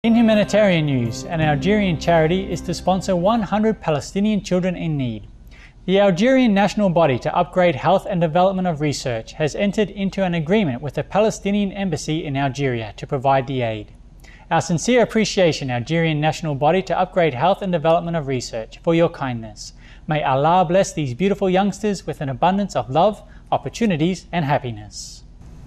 HUMANITARIAN NEWS - Algerian NGO signs agree sponsoring 100 Palestinian orphans
In humanitarian news, an Algerian charity is to sponsor 100 Palestinian children in need. The Algerian National Body to Upgrade Health and Development of Research has entered into an agreement with the Palestinian embassy in Algeria to provide the aid. Our sincere appreciation, Algerian National Body to Upgrade Health and Development of Research, for your kindness. May Allah bless these beautiful youngsters with an abundance of love, opportunities, and happiness.